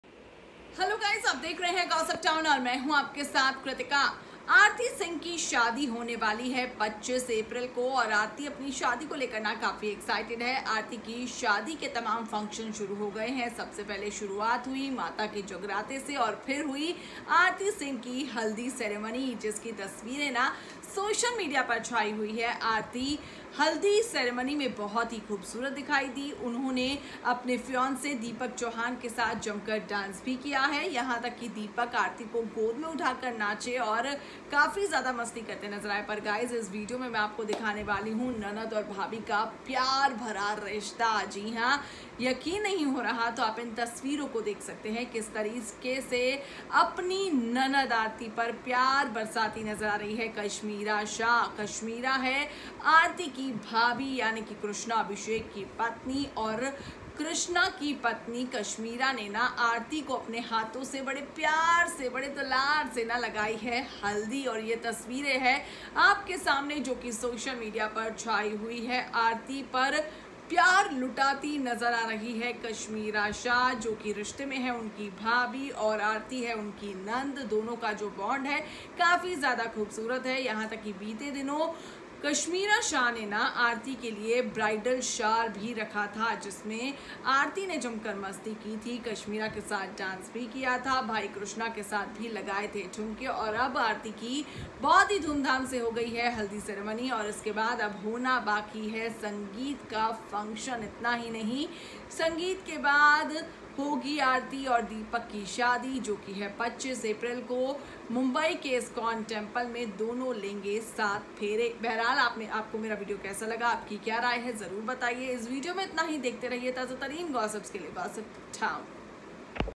Arti Singh को भाभी Kashmira Shah ने लगाई हल्दी
arti  Singh haldi ceremony. Watch the video to know more about this.